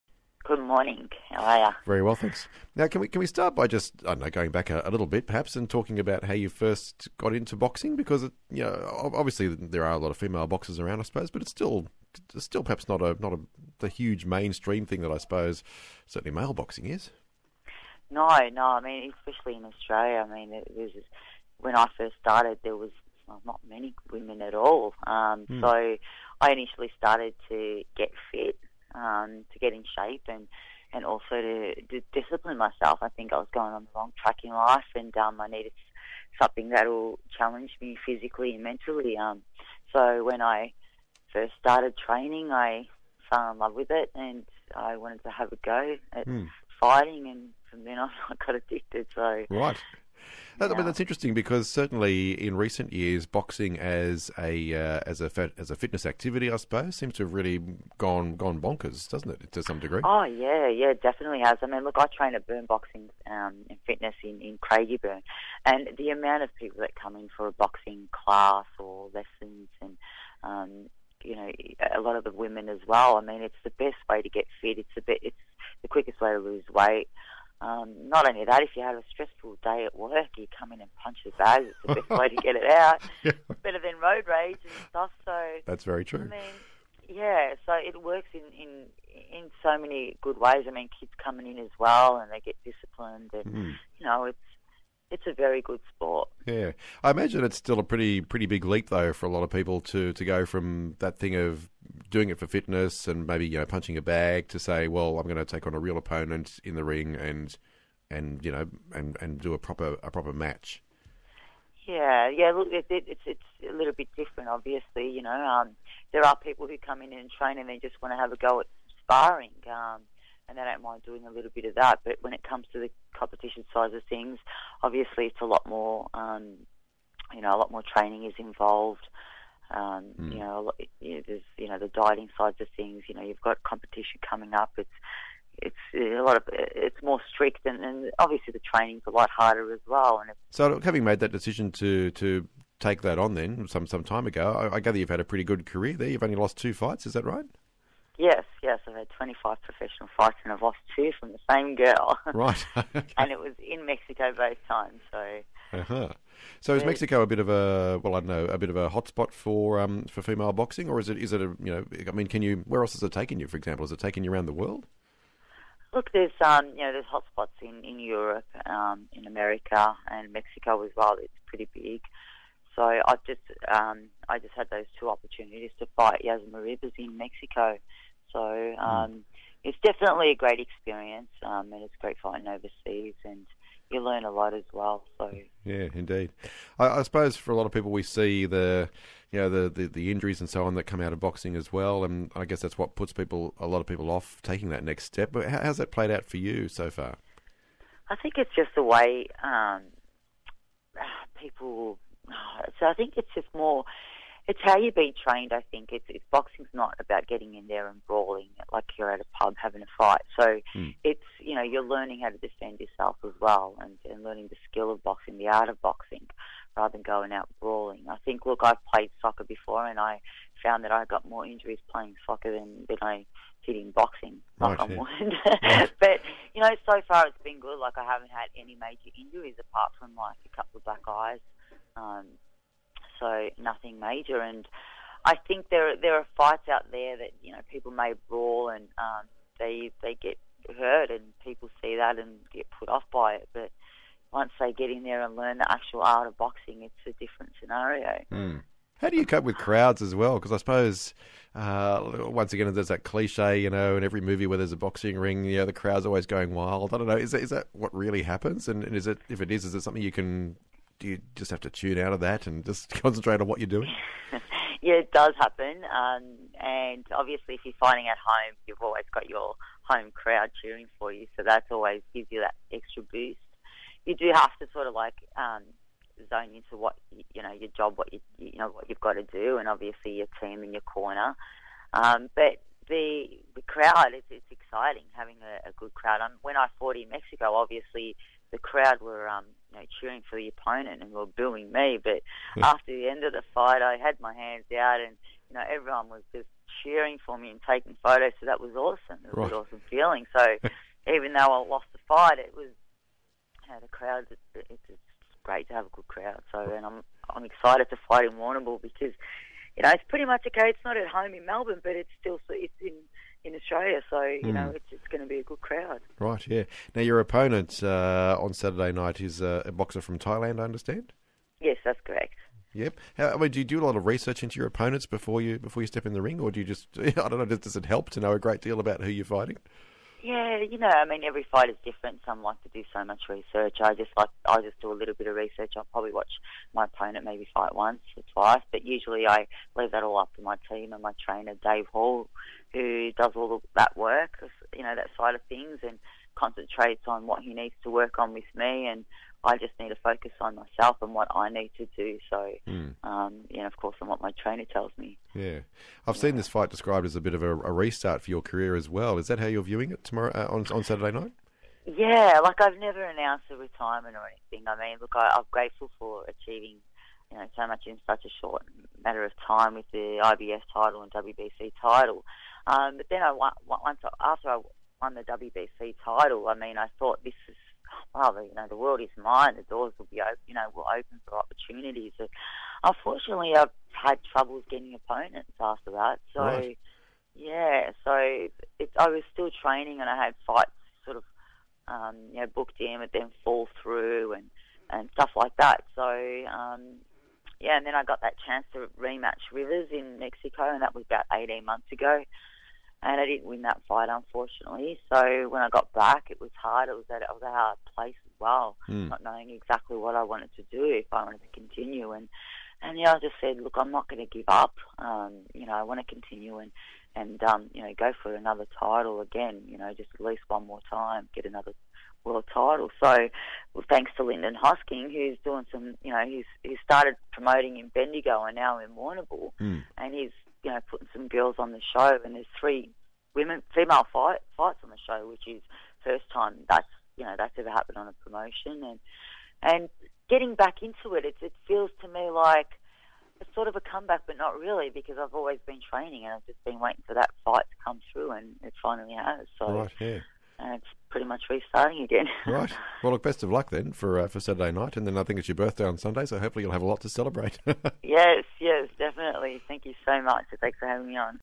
Susie Q Ramadan Interview with Jeremy Lee from ABC Radio South West
Susie Q Ramadan pre-fight interview with host Jeremy Lee from ABC Radio South West before her comeback fight this weekend in Warrnambool (9/4/16).